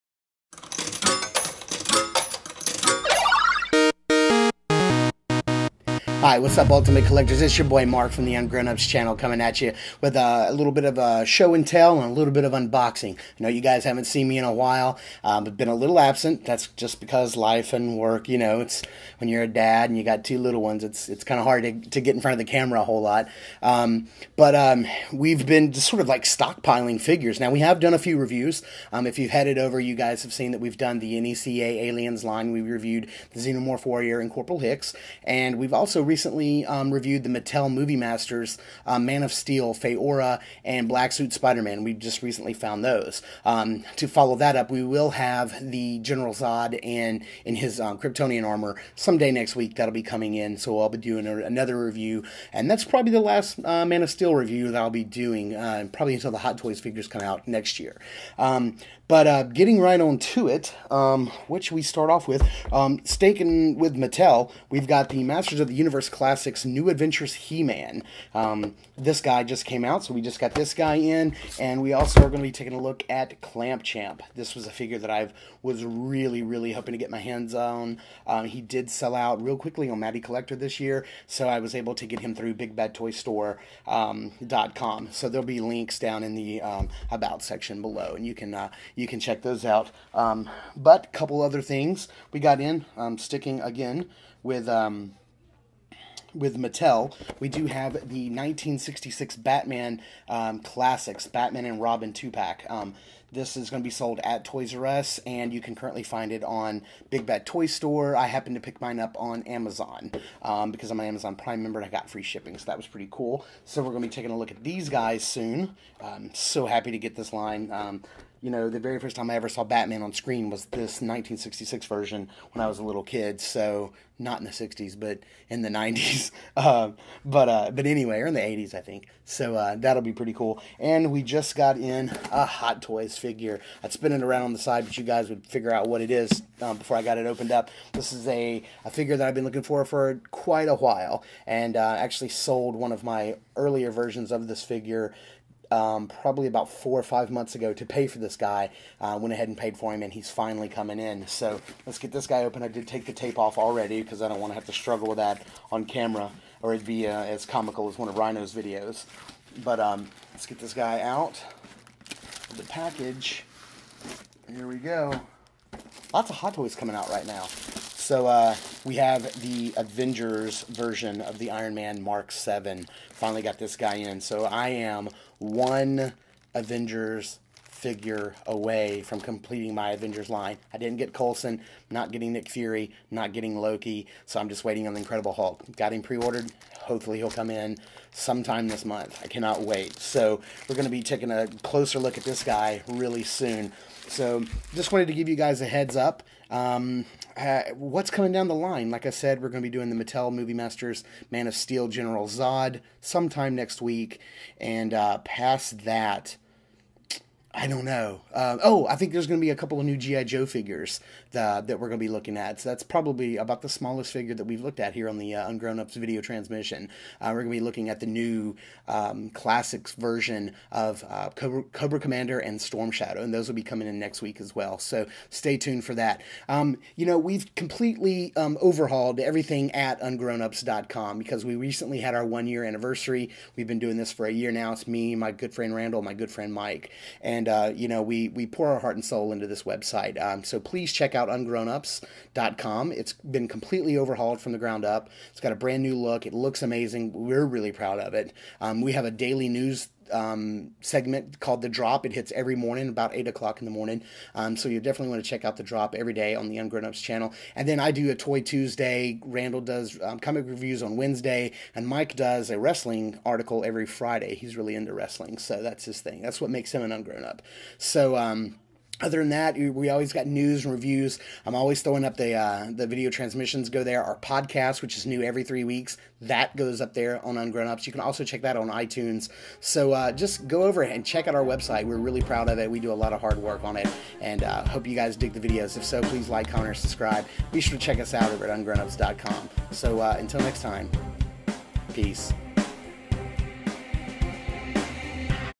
Ungrownups Video Transmission: Hot Toys MMS ... Batman 1966, He-Man and Clamp Champ
Welcome to the Ungrownups Video Transmision! Be sure to check out our site and podcast and be sure to join the Ungrownup army